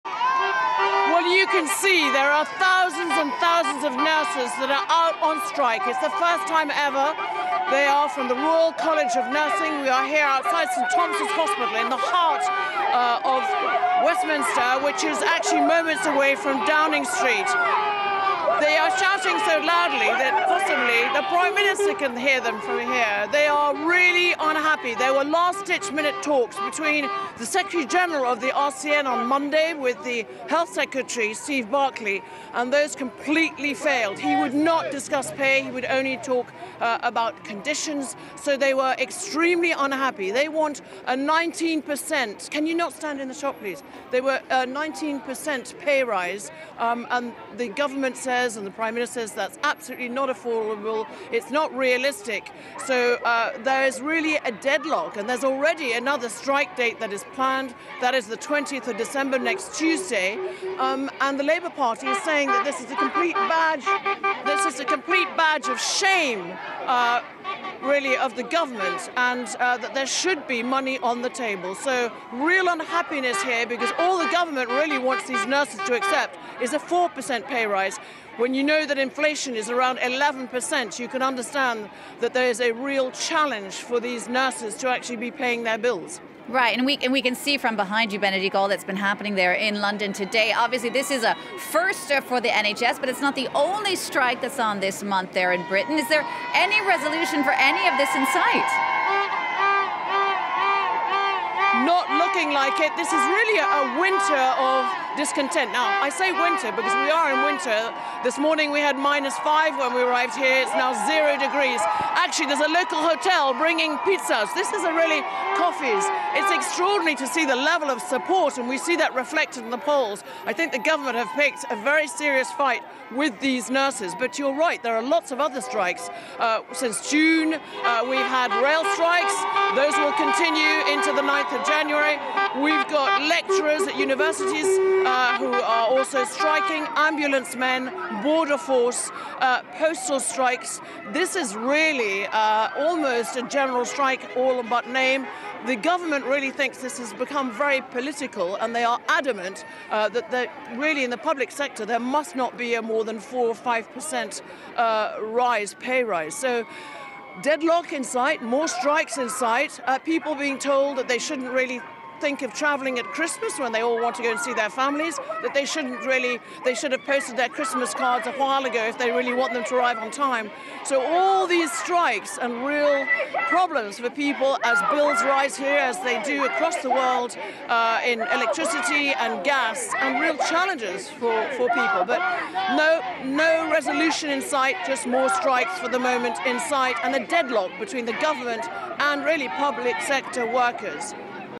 UK nurses strike for better wages and conditions for first time • FRANCE 24 English
UK nurses on Thursday staged an unprecedented one-day strike as a "last resort" in their fight for better wages and working conditions, despite warnings it could put patients at risk. FRANCE 24 correspondent Bénédicte Paviot who is on the ground outside St Thomas' Hospital in London says the nurses "want a 19% pay rise, and the government says that's absolutely not realistic". Another strike is planned for 20 December.